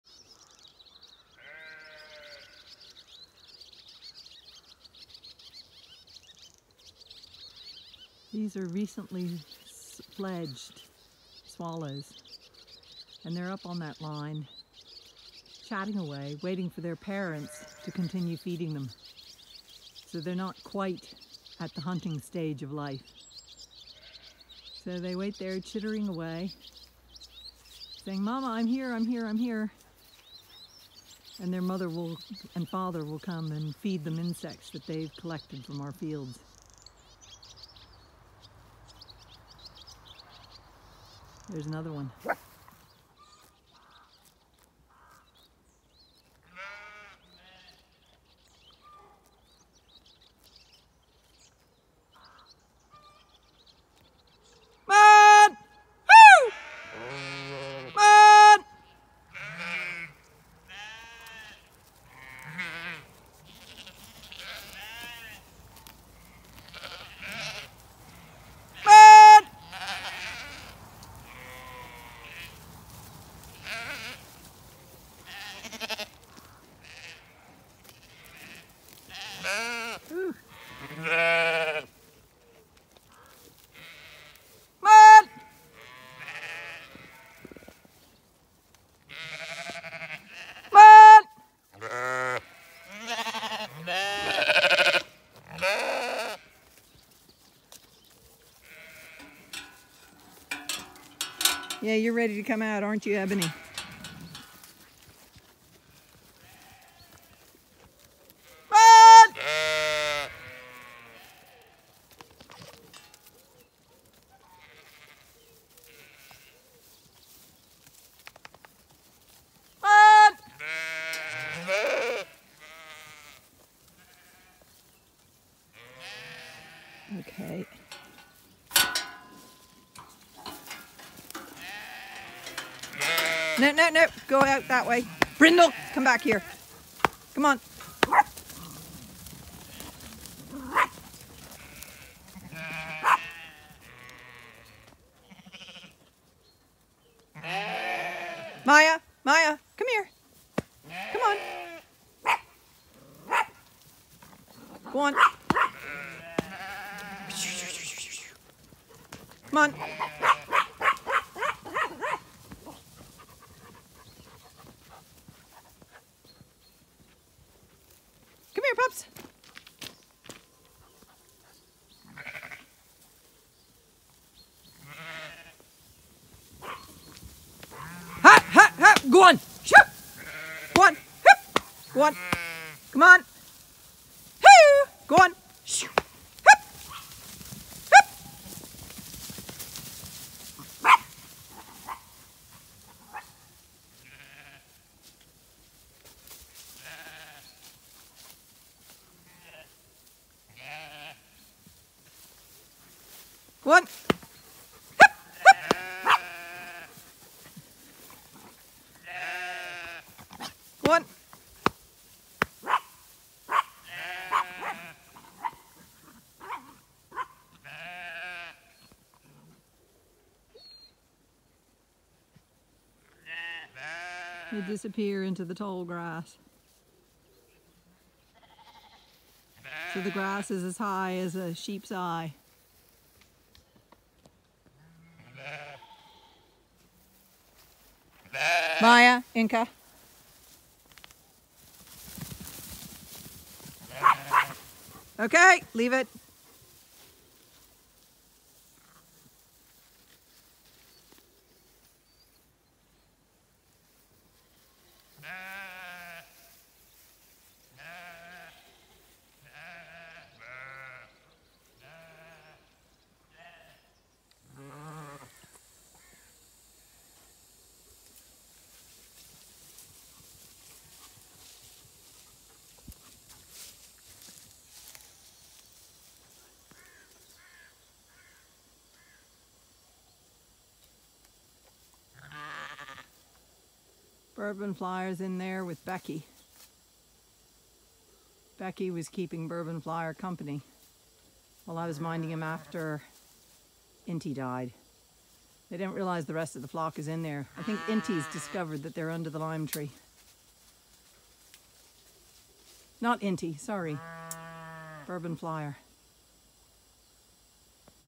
Swallow’s second batch of fledged chick watch as I move sheep
Keen to move into fresh pasture. Inca helps herd in stragglers and finds it thirsty work.